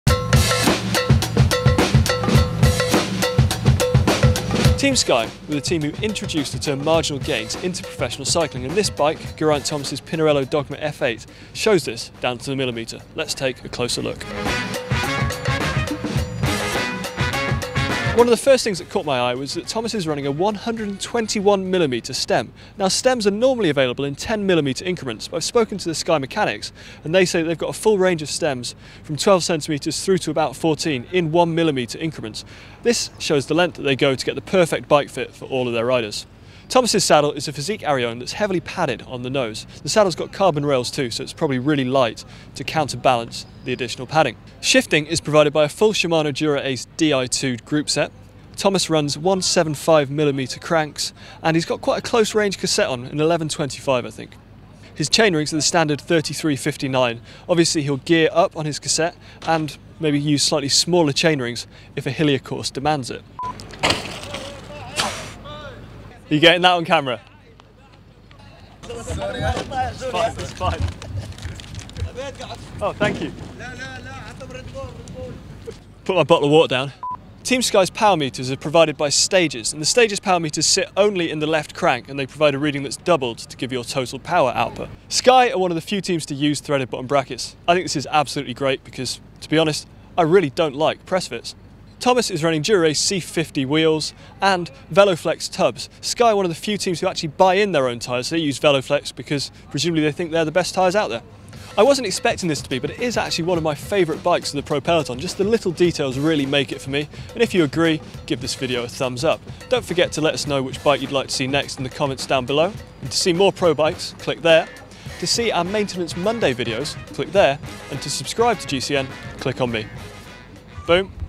Geraint Thomas' Pinarello Dogma F8
Tom takes a closer look at the Pinarello Dogma F8 of Team Sky's Geraint Thomas.

Team Sky are renowned for their attention to the little details, and this is no more apparent in the stem size - Thomas runs a 121mm PRO stem - fitted quite literally to the millimetre.

Shifting comes from a full Shimano Dura-Ace Di2 groupset, with Shimano also providing their full range of wheels, with Dura-Ace pedals to boot.

Topping things off, we have a Fizik saddle - Thomas goes for a nicely padded Arione.

The power meter comes from Stages. Team Sky are the only team in the WorldTour so far to use the left-crank based system.